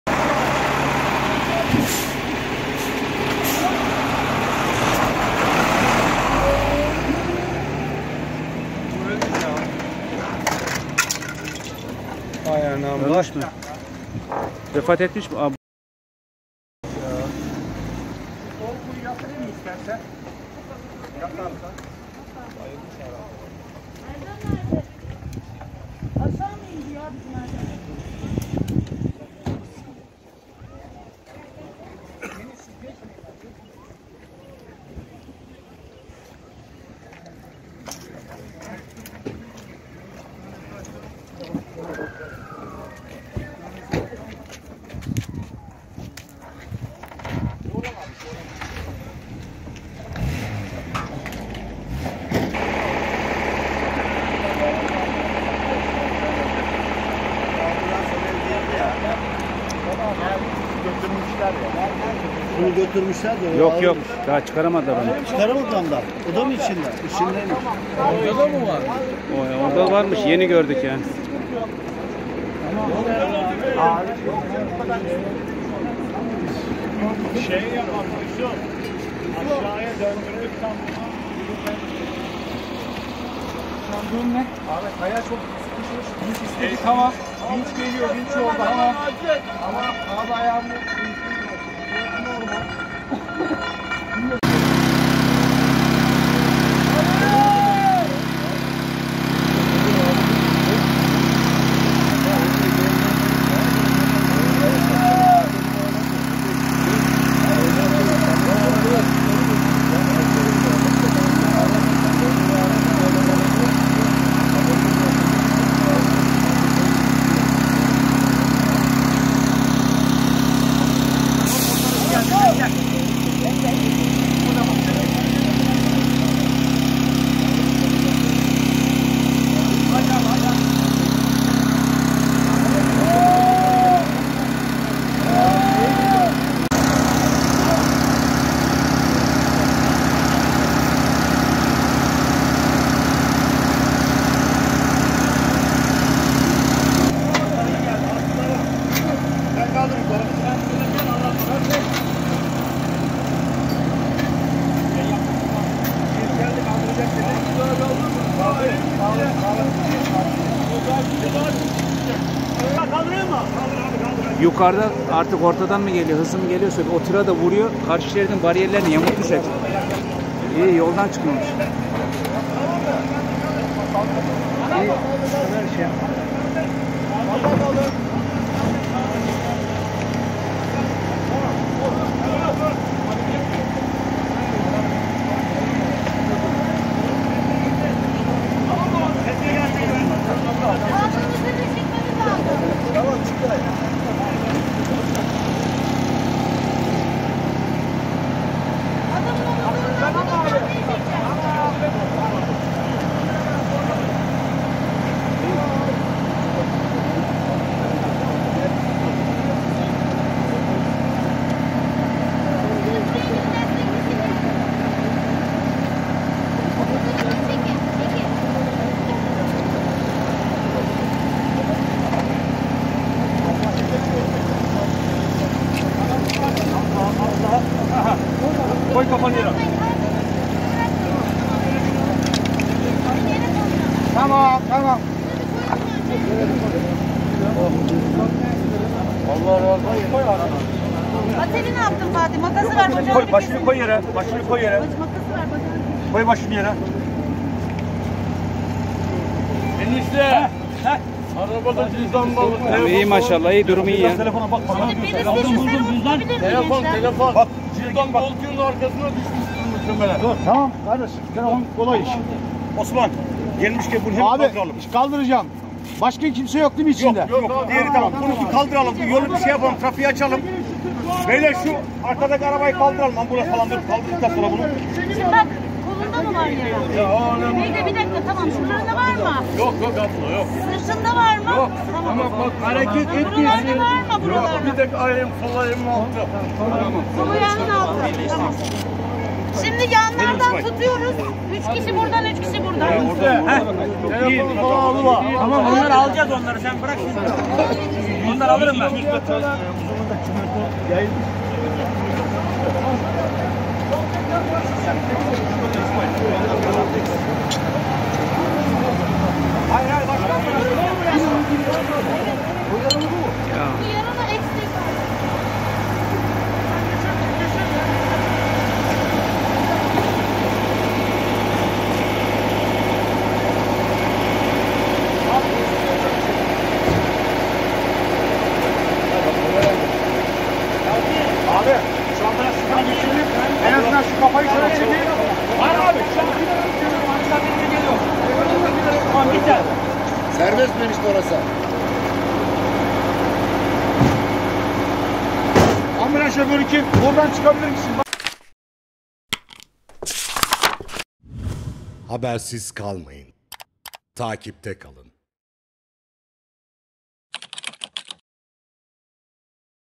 Bilecik Gülümbe rampalarında 2 tır çarpıştı
Meydana gelen trafik kazasında 1 sürücü hayatını kaybetti, 1 sürücü yaralandı.